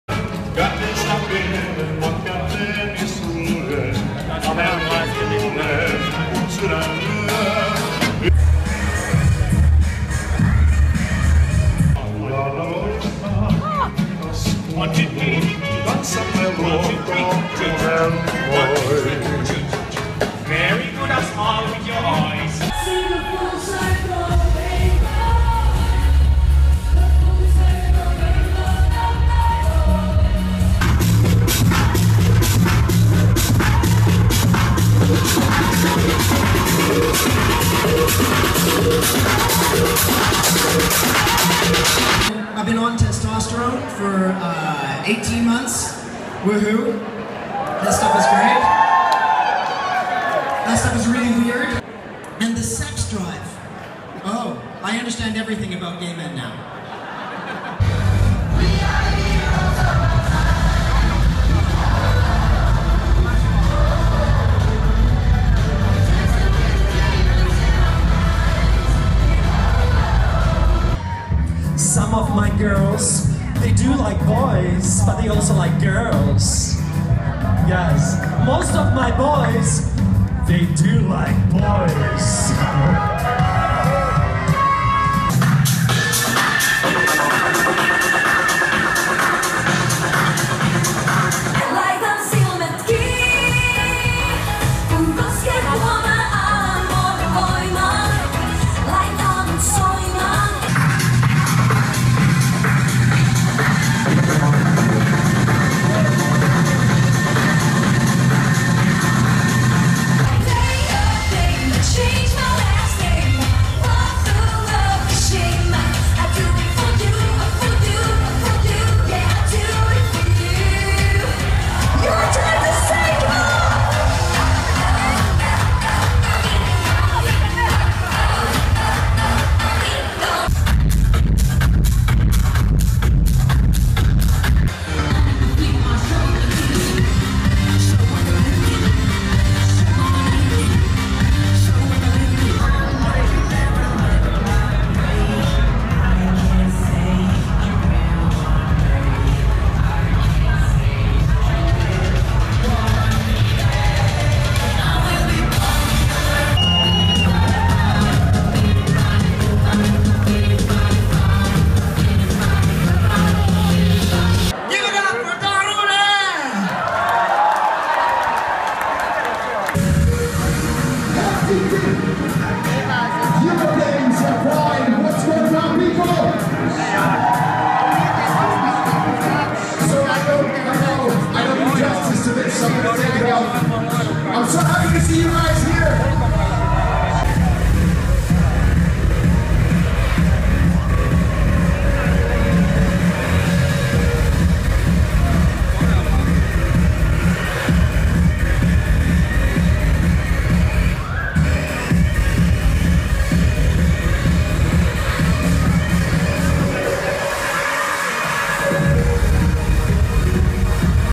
Thank you for the best Pride party ever! LOVE WINS!!!!
Thank you everyone for making CLOSINGPARTYHEL the biggest lgbtiq party ever in the Nordics to date! Here's a little recap of what video I caught on my phone last night.

My company Happy Endings Oy produced the content and marketing of the event for DTM. I'm so proud! Thank you everyone, all party goers and team members and performers for making this happen.